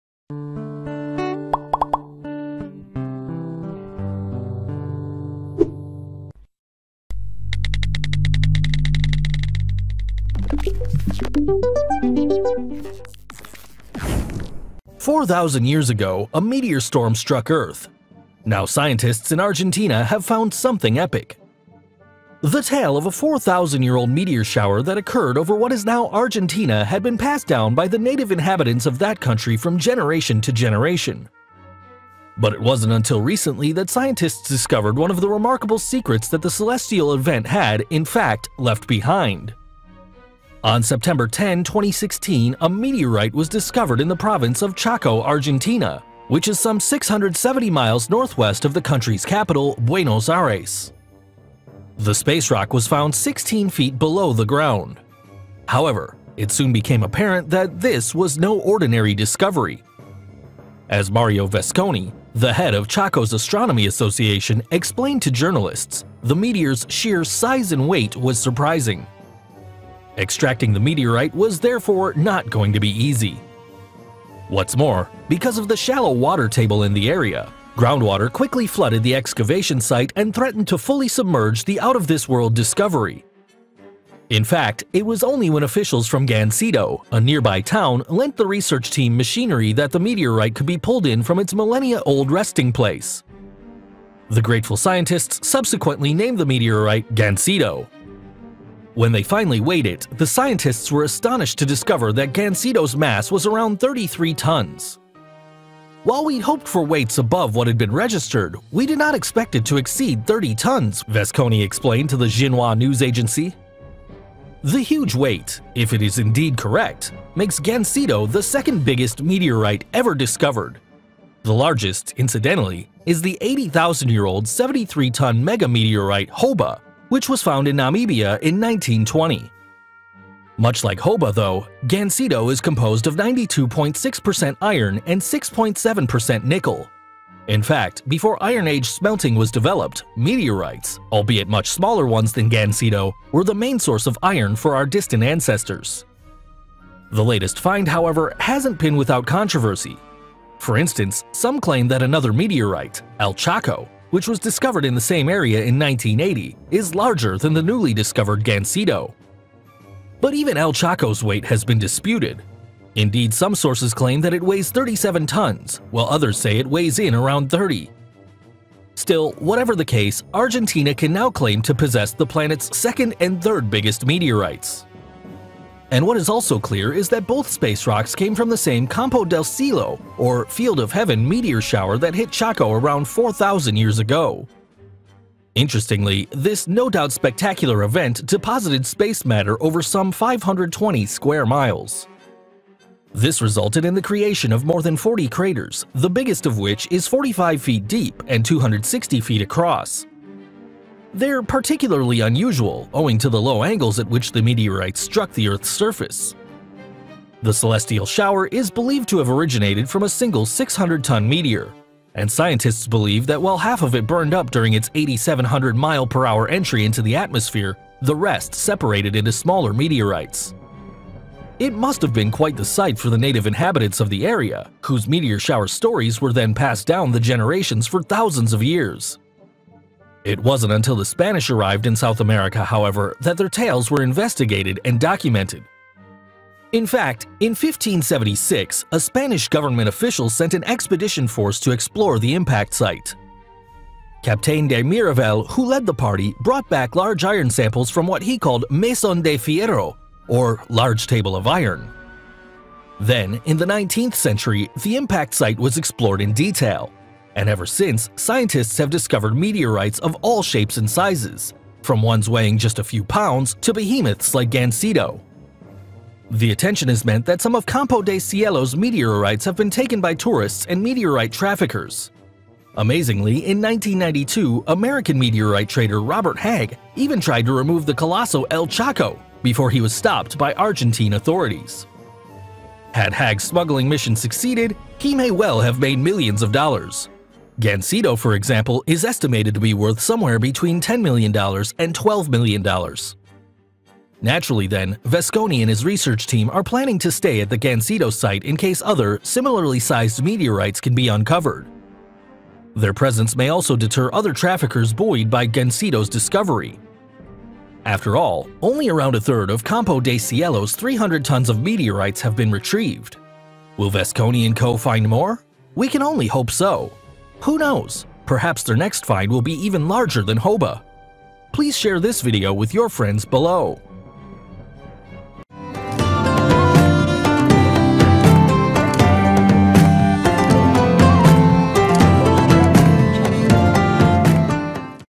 4,000 Years Ago A Meteor Storm Struck Earth Now Scientists In Argentina Have Found Something Epic
The tale of a 4,000-year-old meteor shower that occurred over what is now Argentina had been passed down by the native inhabitants of that country from generation to generation. But it wasn’t until recently that scientists discovered one of the remarkable secrets that the celestial event had, in fact, left behind.On September 10, 2016, a meteorite was discovered in the province of Chaco, Argentina, which is some 670 miles northwest of the country’s capital, Buenos Aires. The space rock was found 16 feet below the ground.